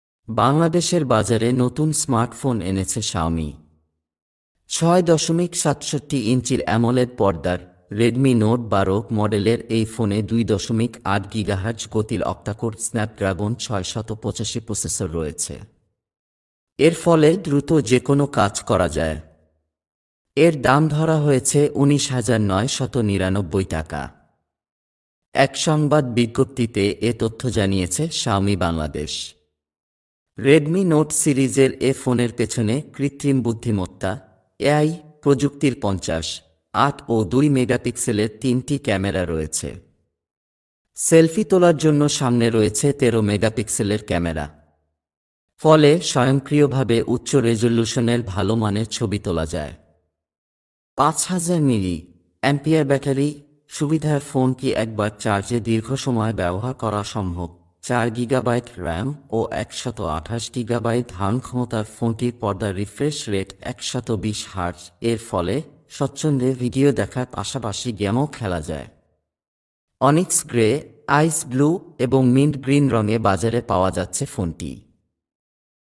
শাওমির নতুন স্মার্টফোন 'রেডমি নোট ১২' _ Xiaomi Redmi Note 12
বাংলাদেশের বাজারে নতুন স্মার্টফোন এনেছে শাওমি। ৬ দশমিক সাতষট্টি ইঞ্চির অ্যামোলেড পর্দার ‘রেডমি নোট বারো’ মডেলের এই ফোনে ২ দশমিক ৮ গিগাহার্জ গতির অক্টাকোর স্ন্যাপড্রাগন ৬ শত পঁচাশি প্রসেসর রয়েছে। এর ফলে দ্রুত যেকোনো কাজ করা যায়। এর দাম ধরা হয়েছে উনিশ হাজার ৯ শত নিরানব্বই  টাকা। এক সংবাদ বিজ্ঞপ্তিতে এ তথ্য জানিয়েছে শাওমি বাংলাদেশ।